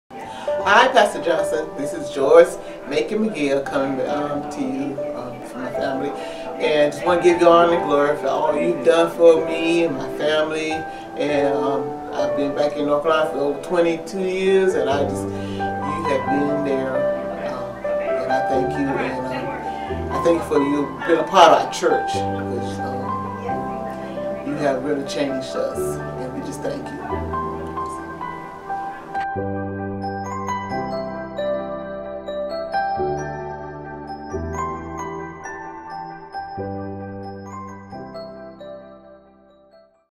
Pastor Appreciation Month - Day 25 Gethsemane SDA Church, Raleigh, NC
Church leaders and members extend their appreciation to Dr. Larry Johnson, Senior Pastor at Gethsemane SDA Church in Raleigh, North Carolina during Pastor Appreciation Month.